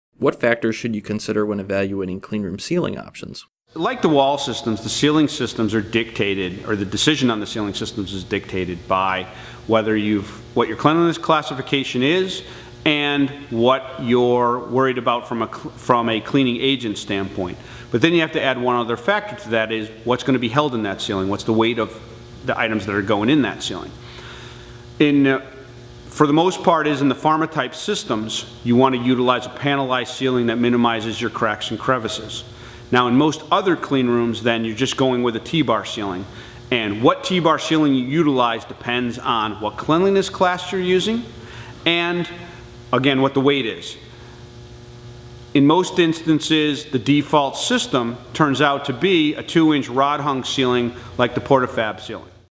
Clean Room Design FAQs: Considerations for Choosing a Cleanroom Ceiling System
QUESTION: What factors should you consider when evaluating cleanroom ceiling systems?

Making a decision on cleanroom ceiling systems, like the wall systems, is dictated by what your cleanliness classification is and what you’re worried about from a cleaning agent standpoint. But then you have to add one more factor to that, which is how much weight the ceiling will need to support.

Commonly with pharmaceutical clean room systems, you’ll want to use panelized ceiling systems that minimize the amount of available cracks and crevices to prevent microbial growth. In most other cleanroom systems you’ll be going with a T-bar ceiling. Determining the type of T-bar ceiling you use depends on the cleanliness classification and the weight of the ceiling. In most instances, the default system turns out to be a 2-inch rod hung ceiling, like the common PortaFab ceiling system.